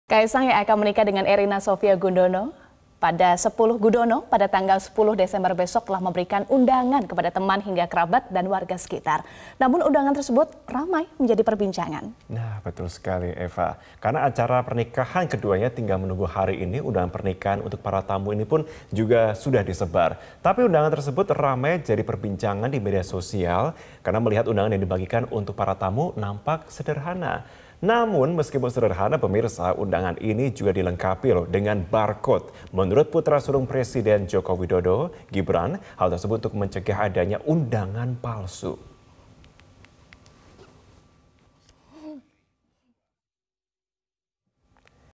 Tampak Sederhana, Undangan Pernikahan Kaesang Erina Jadi Perbincangan Warganet
MetroTV, Kaesang yang akan menikah dengan Erina Sofia Gundono pada tanggal 10 Desember 2022, telah memberikan undangan kepada teman hingga kerabat dan warga sekitar.

Walaupun sederahana , undangan ini juga dilengkapi dengan barcode. Menurut Putra Sulung Presiden Jokowi, Gibran, hal tersebut untuk mencegah adanya undangan palsu.